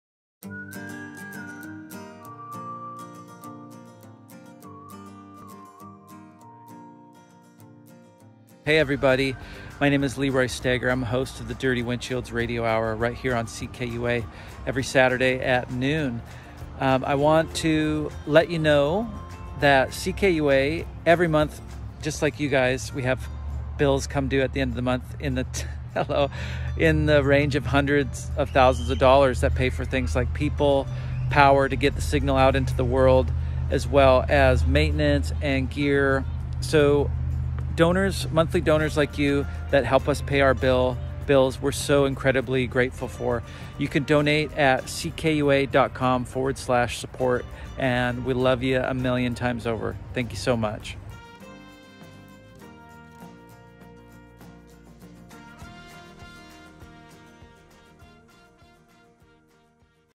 Did you know being a monthly donor is the biggest difference you can make?
Monthly Donors are a stable base of monthly income for CKUA. We pay monthly bills just like you, which, for CKUA, adds up to hundreds of thousands of dollars. Monthly donors give us a stable monthly income so we can pay our bills on time. And that's one of the million reasons why we are grateful when you become a monthly donor